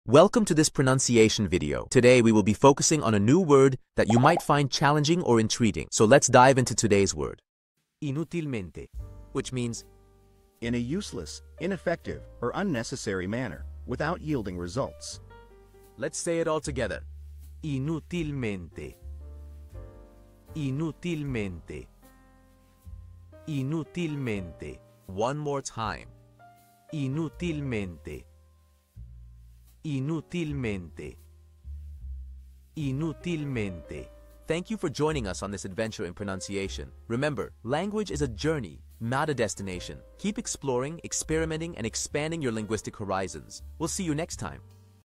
How to pronounce Inutilmente
Master the Pronunciation of 'Inutilmente Which means unnecessarily' - which means : In A Useless, ineffective, or unnecessary manner;Without Yielding Results. 📝 with @PizzaPronunciation 🎓🗣️ - [Your Guide to Italian]

Learn to pronounce 'Inutilmente' perfectly in Italian with our easy and engaging tutorial by @PizzaPronunciation! 📖📽️ In this comprehensive video, we provide you with native speaker examples 🗣️👂 and phonetic breakdowns 🎵 to ensure you get the pronunciation just right.

How to say Uselessly in Italian